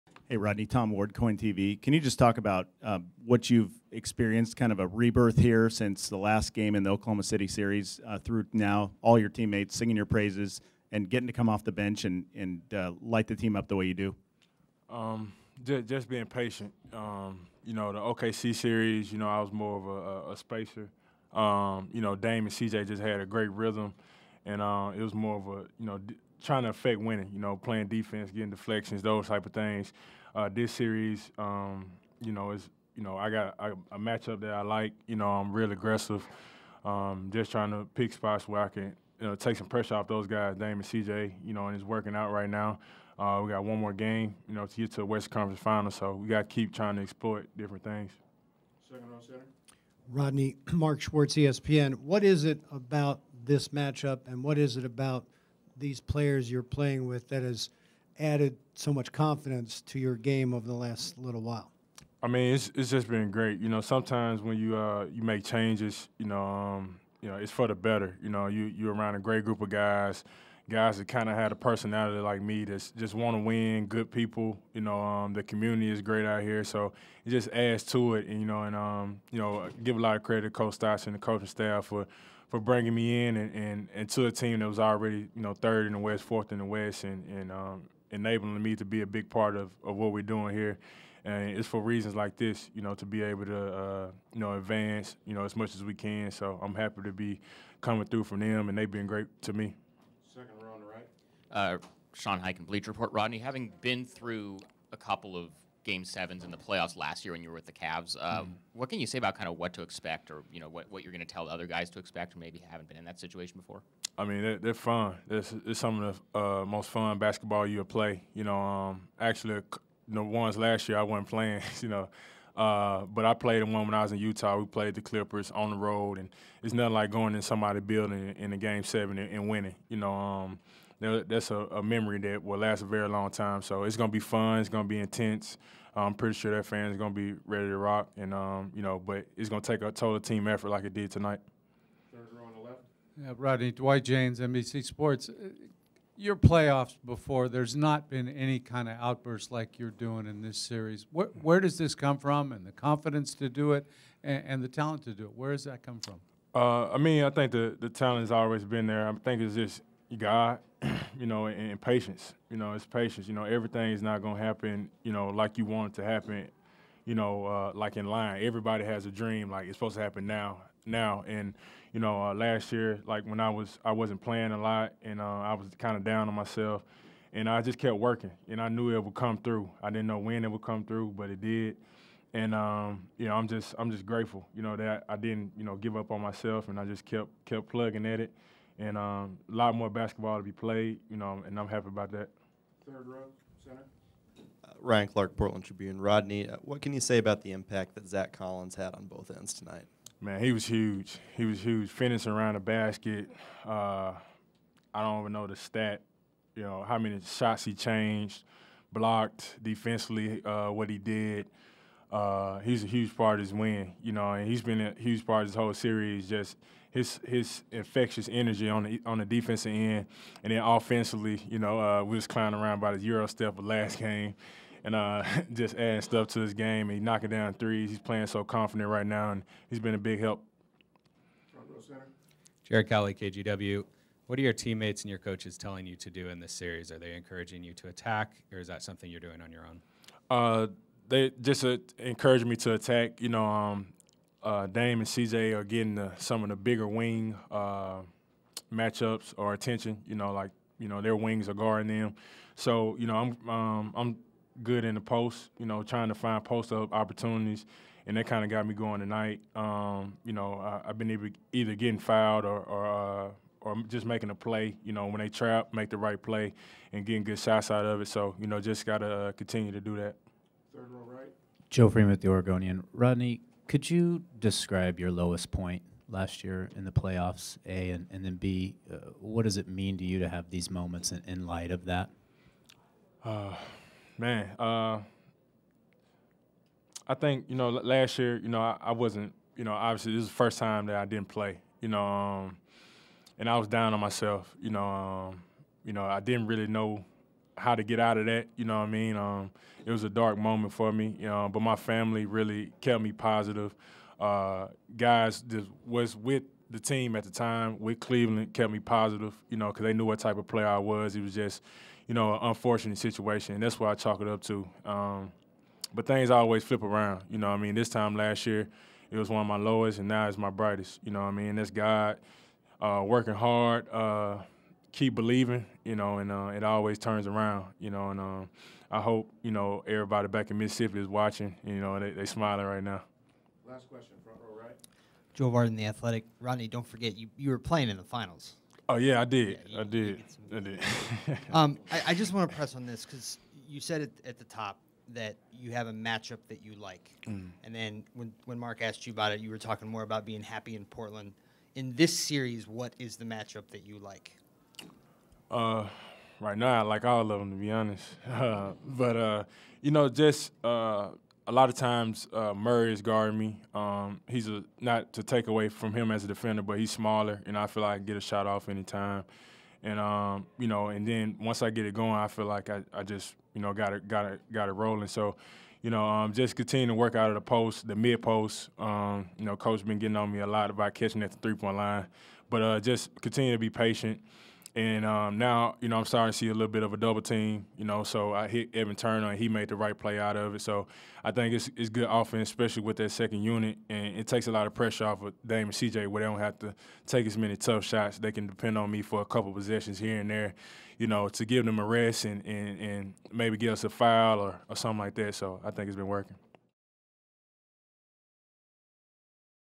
Rodney Hood: "There's nothing like going into someone's building in Game 7 and winning"
Rodney Hood speaks to media after scoring a postseason career high 25 points in the Game 6 win over the Nuggets on Thursday.